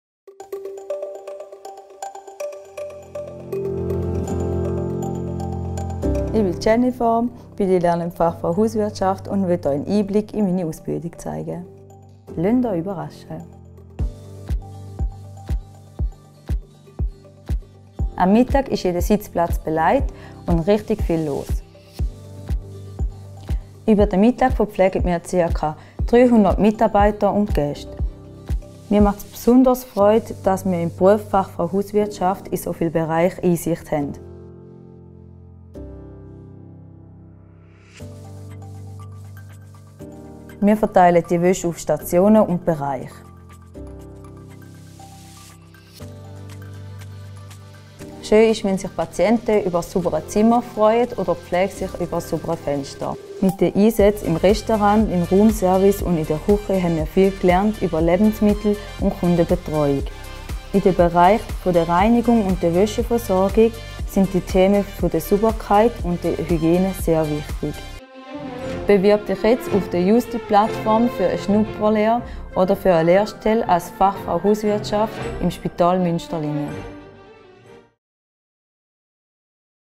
Lehre als Fachmann/-frau Hauswirtschaft EFZ bei der Spital Thurgau AG (Münsterlingen)
Ein Einblick in den Alltag einer Lehrstelle bei der Spital Thurgau AG in Münsterlingen, Thurgau, als Fachmann/-frau Hauswirtschaft EFZ